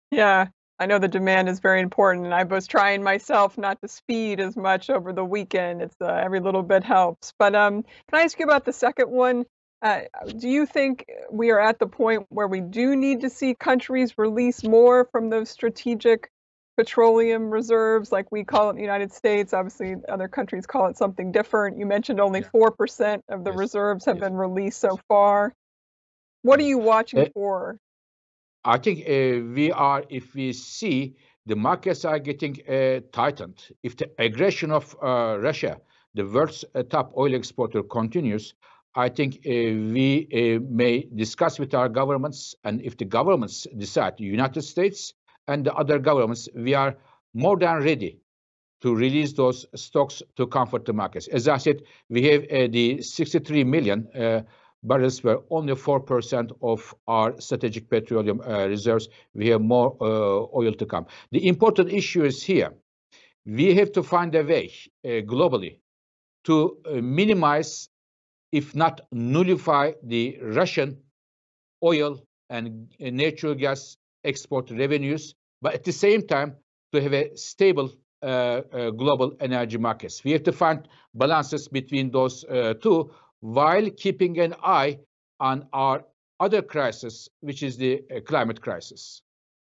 Fatih Birol says IEA is ‘more than ready’ to release stockpiled oil to ease energy market prices
“If the aggression of Russia… continues, we may discuss with our governments and if the governments decide, the United States and the other governments, we are more than ready to release those stocks to comfort the markets… We have to find a way globally to minimize if not nullify the Russian oil and natural gas revenues, but at the same time to have a stable global energy markets.” - Fatih Birol